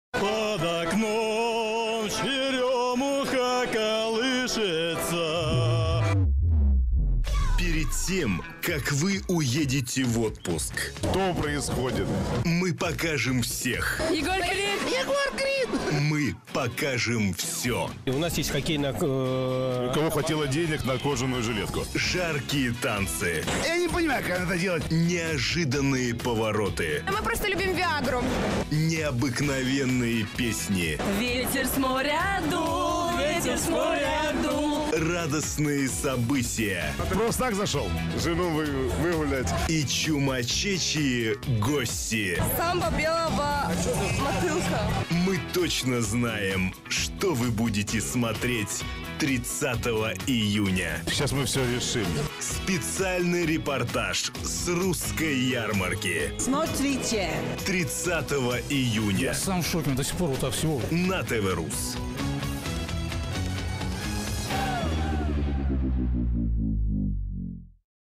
Анонс Специального репортажа с "Русской ярмарки" 2017. Телеканал TVRus
Всё самое интересное, самое захватывающее, самое необыкновенное, что было на "Русской ярмарке", смотрите 30 июня в 20:00 в специальном репортаже на телеканале TVRus! Мы покажем всех! Мы покажем всё! Приготовьтесь!
Наташа Королёва, Натали, Егор Крид, Потап и Настя Каменских, Шейла Бонник (Бони М), Милан Савич, Таша Милкова, Виктор Матросов, Аня Рай, Юрий Свобода, Саня Беляев, ГОП-СТОП БАНДА и др.